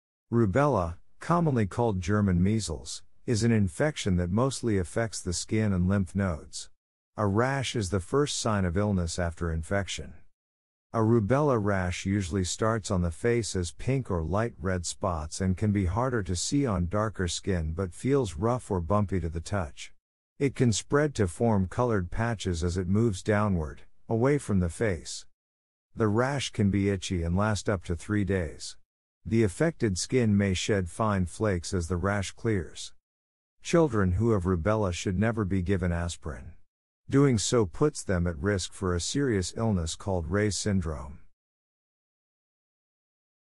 Rubella
Rubella, commonly called German measles, is an infection that mostly affects the skin and lymph nodes. A rash is the first sign of illness after infection..
A rubella rash usually starts on the face as pink or light red spots and can be harder to see on darker skin but feels rough or bumpy to the touch. It can spread to form colored patches as it moves downward, away from the face..
The rash can be itchy and last up to three days. The affected skin may shed fine flakes as the rash clears..
Children who have rubella should never be given aspirin. Doing so puts them at risk for a serious illness called Reye's syndrome..